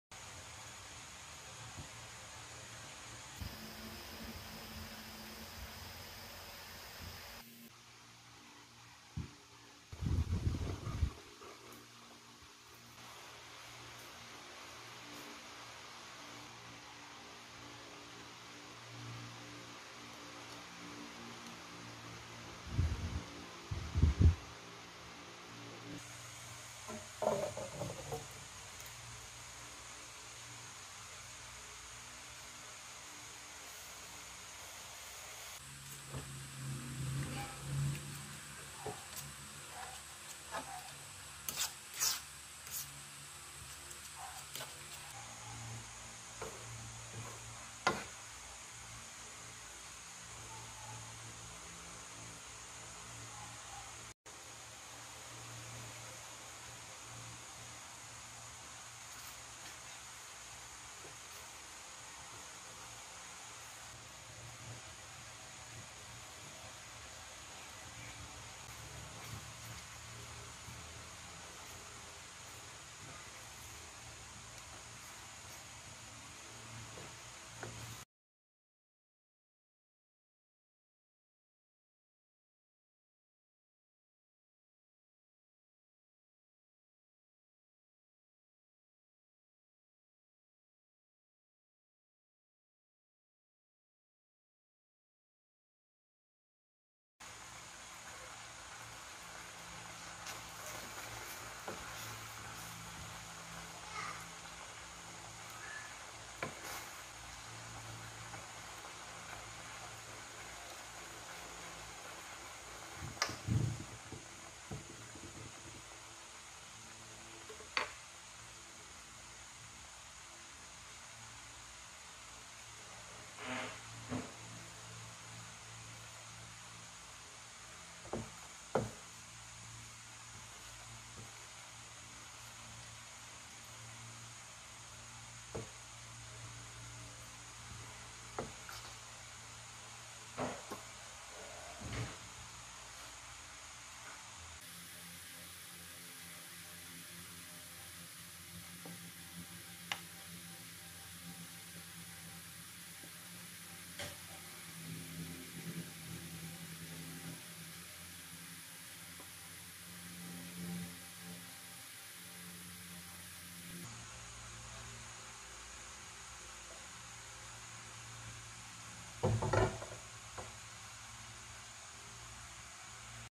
2 ingredients dessert| Canned Red Kidney Beans Paste| Brigit's Kitchen
Itong 2 ingredients dessert na ito ay gawa sa isang latang red kidney beans at isang tasang asukal. Niluto sa mahinang apoy hanggang sa lumapot o naging paste. Masarap na panghimagas at palaman sa mga lutong kakanin. Tara na at subukan nyo!

2 ingredients

1 canned Red Kidney Beans (any brand)
1 cup brown sugar

I hope you enjoy watching my video..see you in my next upload.